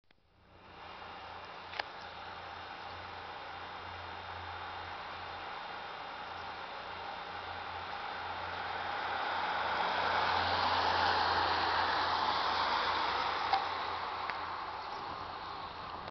ASVH Europarit 2011 Audi TT Cabrio
Etappe 4: Rothenburg - Hellendoorn. Passerende Audi TT Cabrio.